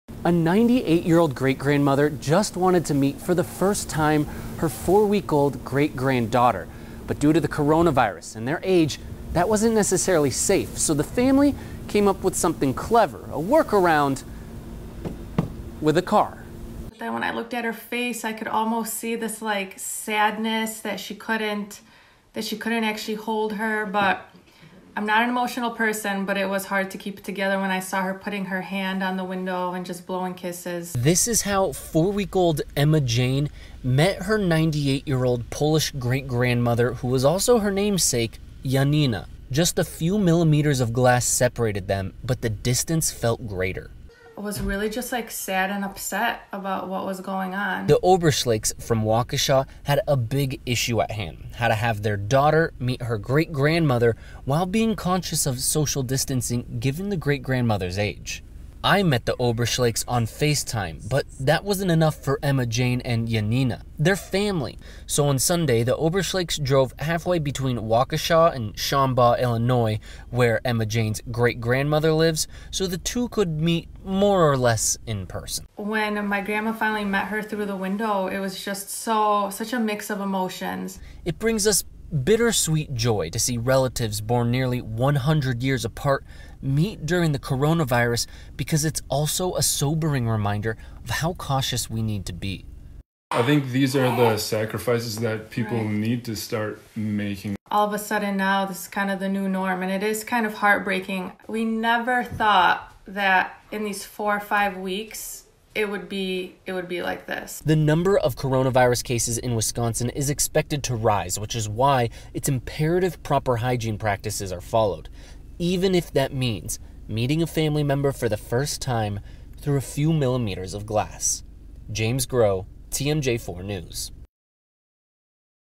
Great grandmother can't hold great granddaughter due to coronavirus
A great-grandmother and great-granddaughter are forced to meet each other through a glass barrier due to the coronavirus.